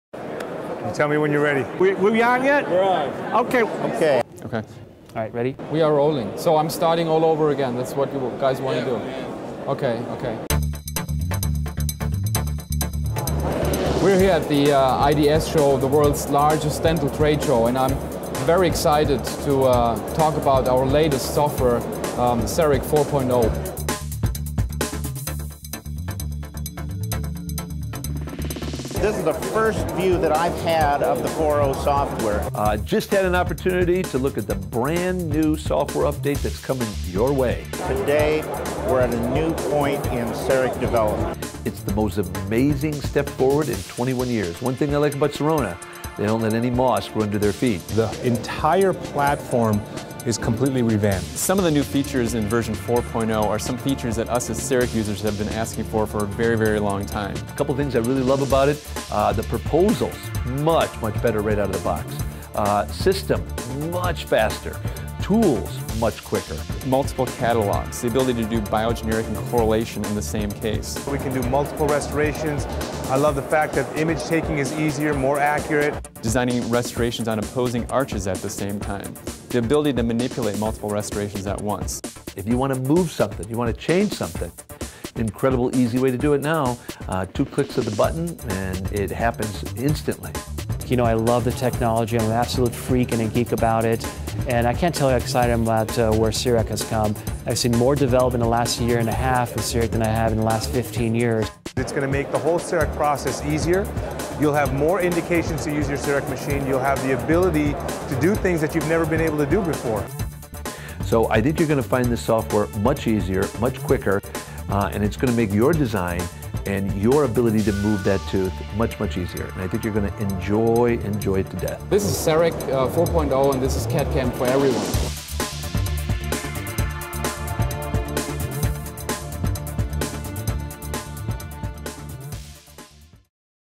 CEREC SW 4.0: Overview from Beta Testers
CEREC 4.0 software introduction from beta testers: Intuitive workflow. New design tools. Photorealistic graphics.